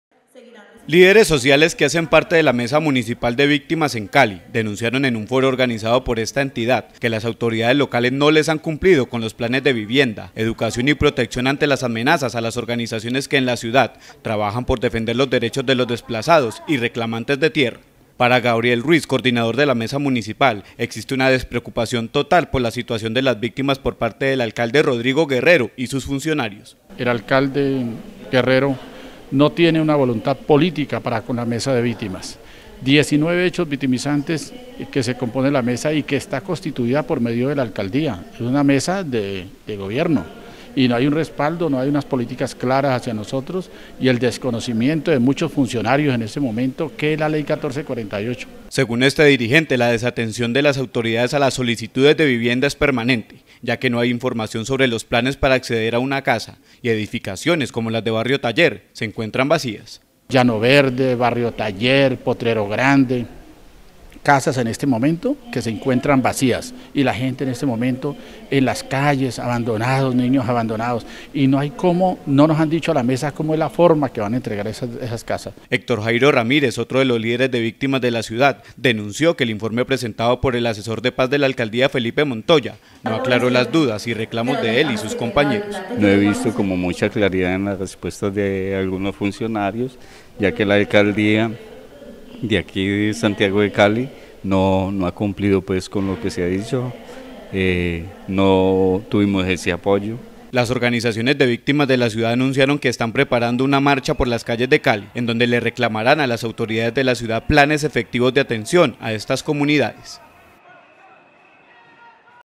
A la deriva y sin ayudas se encuentran víctimas de la violencia en Cali
Líderes sociales que hacen parte de la mesa municipal de víctimas en Cali, denunciaron en un foro organizado por esta entidad que las autoridades locales no les han cumplido con los planes de vivienda, educación y protección ante las amenazas a las organizaciones que en la ciudad trabajan por defender los derechos de los desplazados y reclamantes de tierra.

Para Gabriel Ruiz coordinador de la mesa municipal, existe una despreocupación total por la situación de las víctimas por parte del alcalde Rodrigo Guerrero y sus funcionarios.

Según este dirigente la desatención de las autoridades a las solicitudes de vivienda es permanente, ya que no hay información sobre los planes para acceder a una casa, y edificaciones como las de Barrio Taller se encuentran vacías.

Héctor Jairo Ramírez, otro de los líderes de víctimas de la ciudad denunció que el informe presentado por el asesor de paz de la alcaldía Felipe Montoya no aclaró las dudas y reclamos de él y sus compañeros.

Las organizaciones de víctimas de la ciudad anunciaron que están preparando una marcha por las calles de Cali, en donde le reclamarán a las autoridades de la ciudad planes efectivos de atención a estas comunidades.